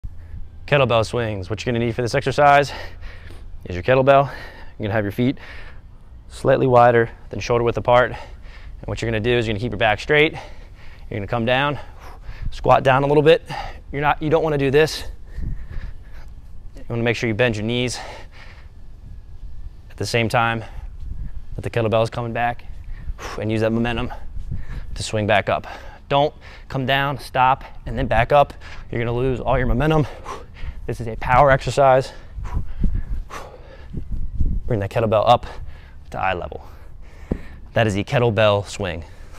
How to do Kettlebell Swings
In this video a personal trainer shows you How to do Kettlebell Swings